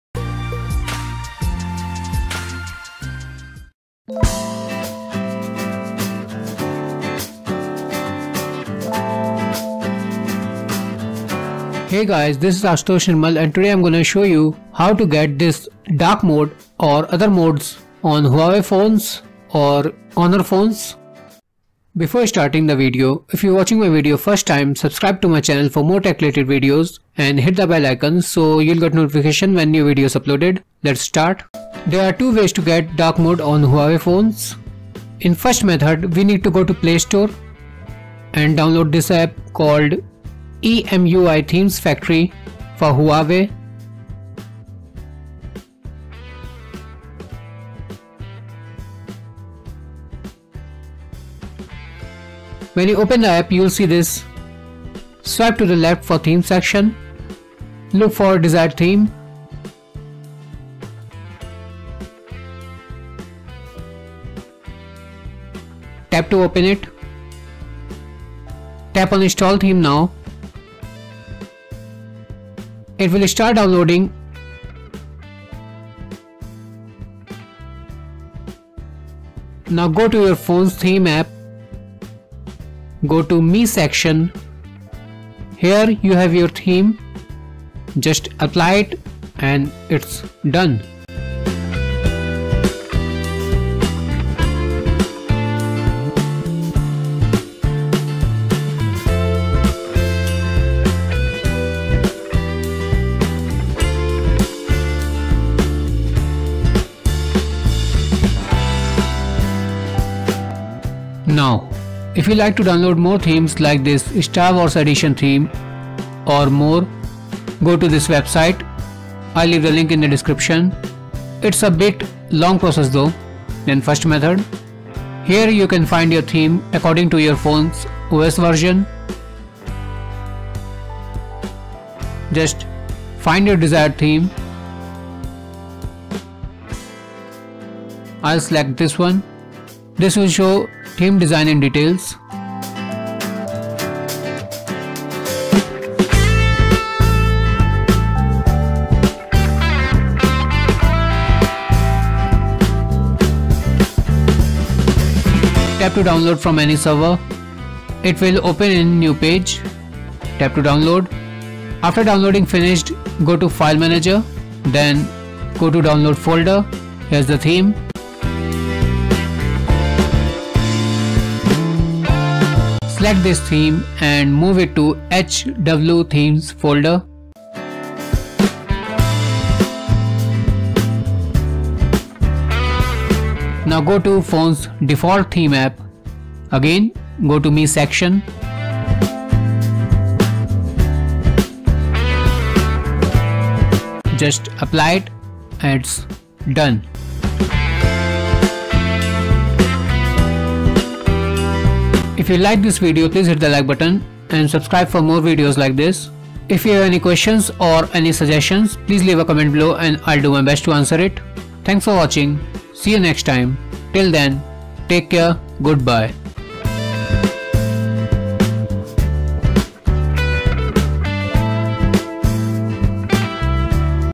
How to get Dark Mode on Huawei Phones or Honor Phones (EMUI 5/8)
Hey guys, this is Ashutosh Nirmal and today I'm gonna show you how to get Dark Mode on Huawei Phones or Honor Phones

How to get Dark Mode on Facebook Messenger (Android)

Flipkart Big Shopping Days
12 Days of Deals (US)

Honor 7X
Honor 8X
Honor 9 Lite